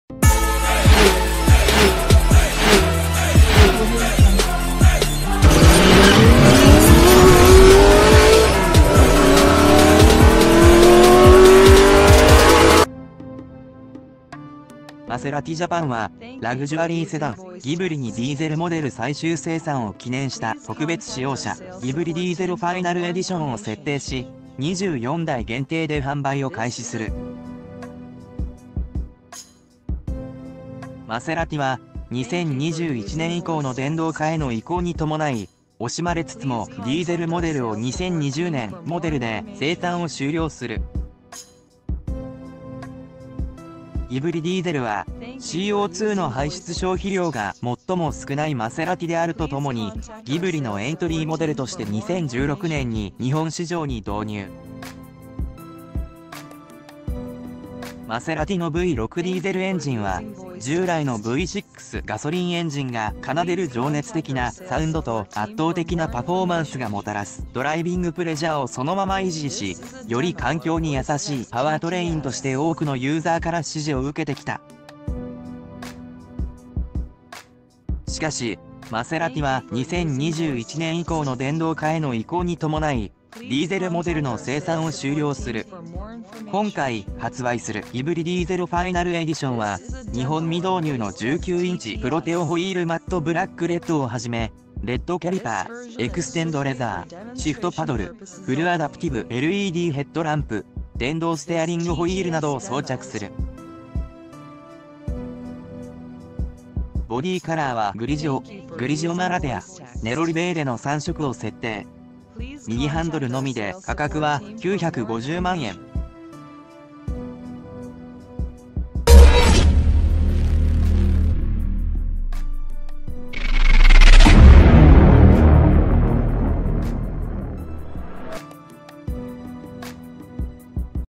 マセラティ ギブリ、ディーゼル ファイナルエディションを発売 日本未導入のホイール装着 ）
,マセラティ ギブリ,マセラティ（Maserati）,
マセラティジャパンは、ラグジュアリーセダン『ギブリ』にディーゼルモデル最終生産を記念した特別仕様車『ギブリ ディーゼル ファイナルエディション』を設定し、24台限定で販売を開始する。 マセラティは、